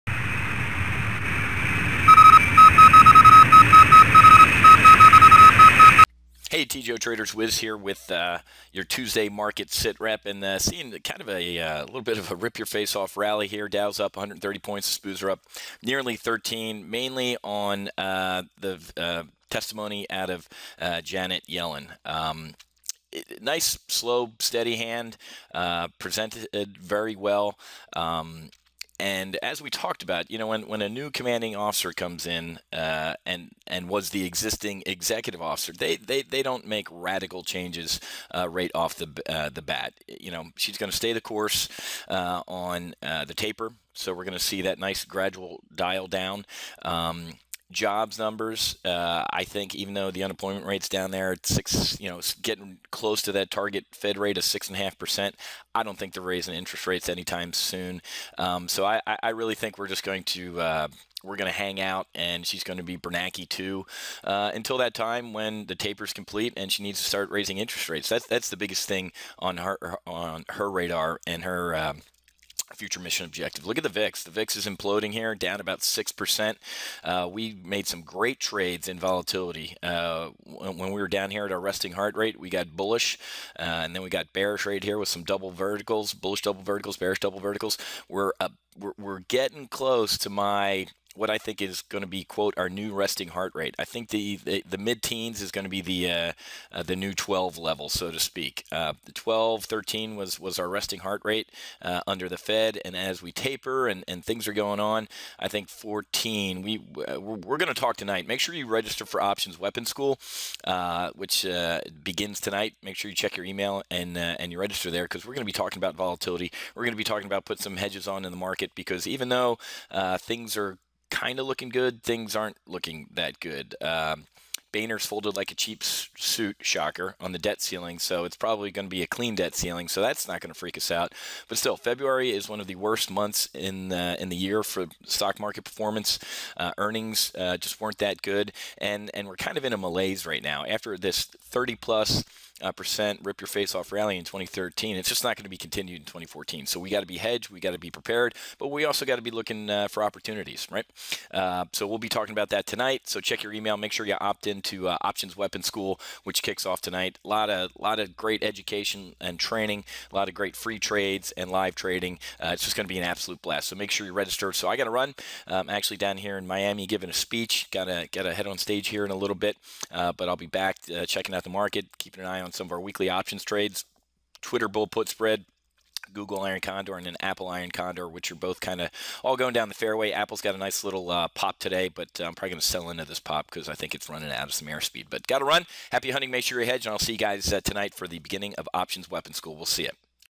[Dispatch] Fed Still Tapers -- Our Model Portfolios Still Profit
In today's Market Situation Report, WCI's Chief Investment Strategist Matthew 'Whiz' Buckley discusses today's rip-your-face-off rally as a reaction to Janet Yellen's testimony on tapering amidst some interesting developments on the VIX.

"February is one of the worst months in the year for stock market performance -- we're kind of in a malaise right now. We got to be hedged and prepared, but also looking for opportunities."